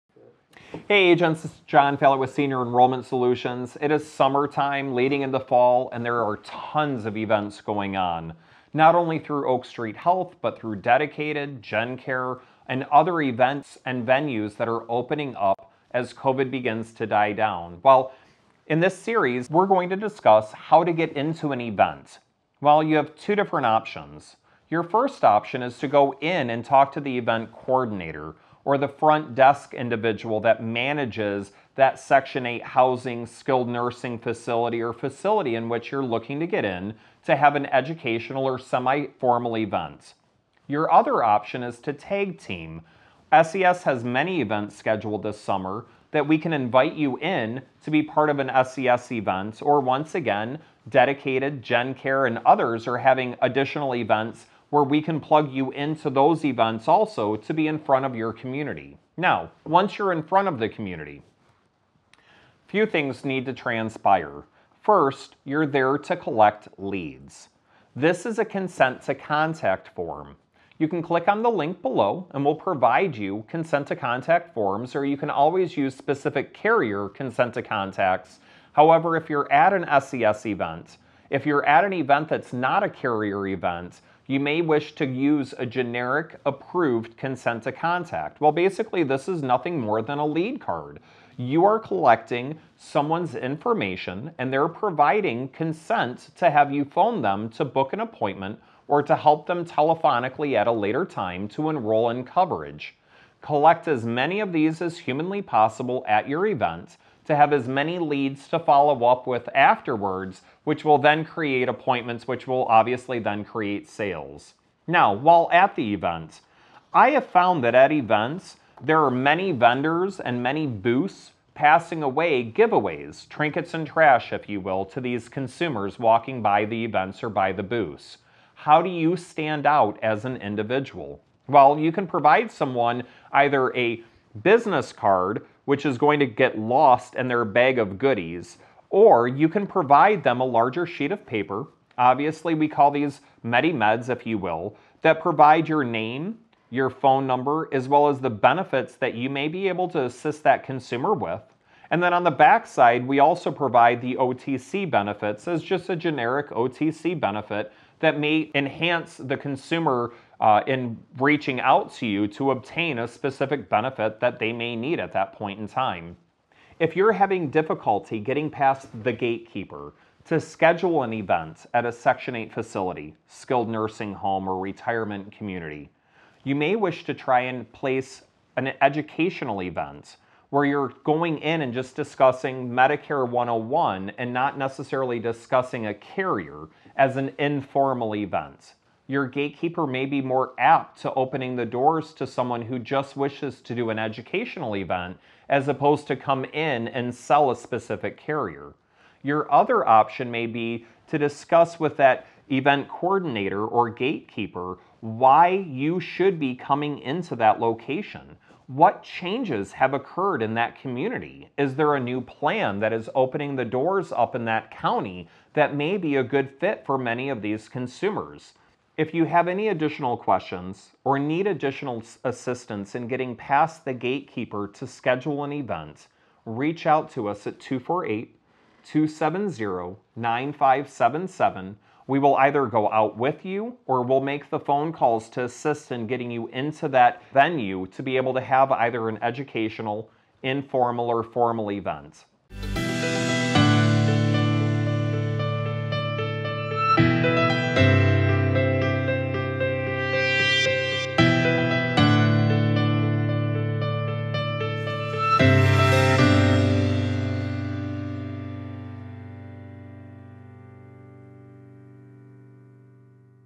How to get into an Event or Facility to offer Medicare | SES Insurance Agent Solutions
How to get into an Event or Facility to offer Medicare
John explains how to get past a gatekeeper, to be allowed to offer your services inside of an event. He also explains some strategies for hosting your own event at a location. If you need help, SES is here to assist you in getting into an event or facility, and can help you in hosting your own event.

Senior Enrollment Solutions
Insurance Solutions for Agents
Medicare | Life | Retirement